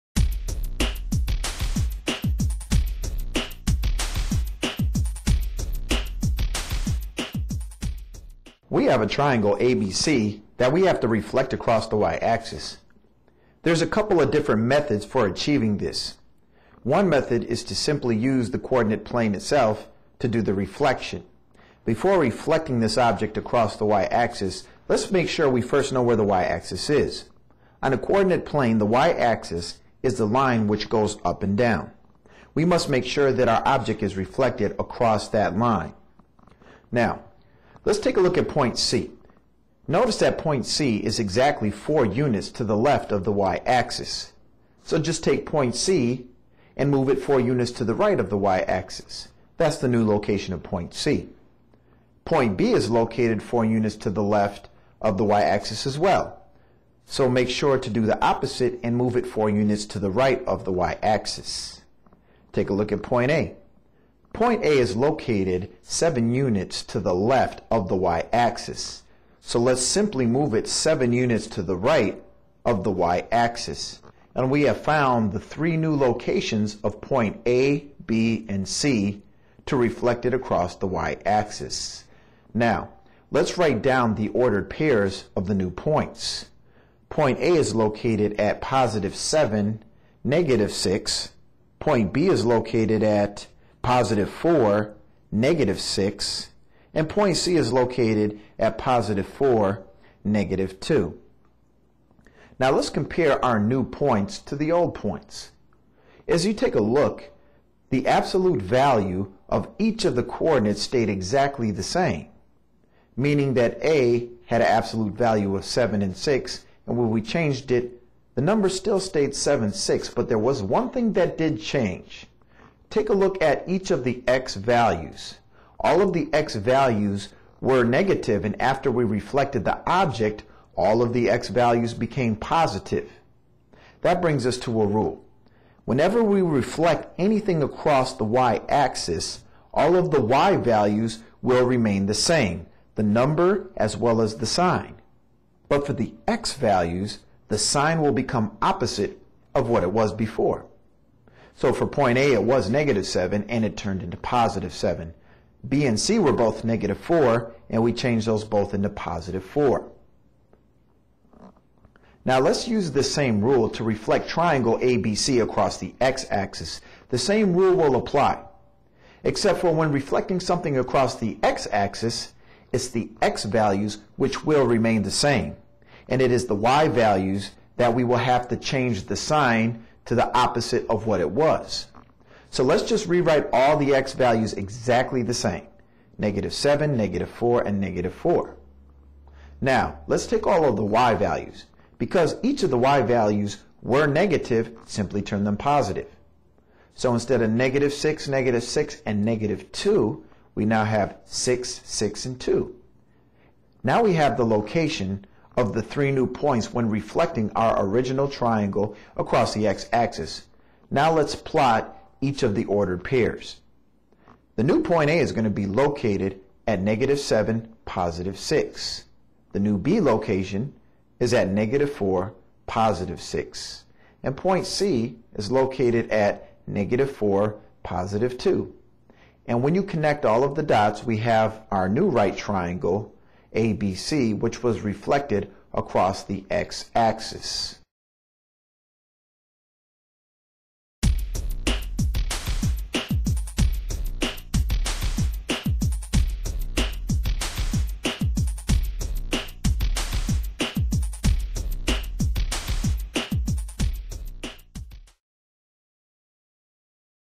Transformations - Reflection
Review the rules for performing a reflection across an axis.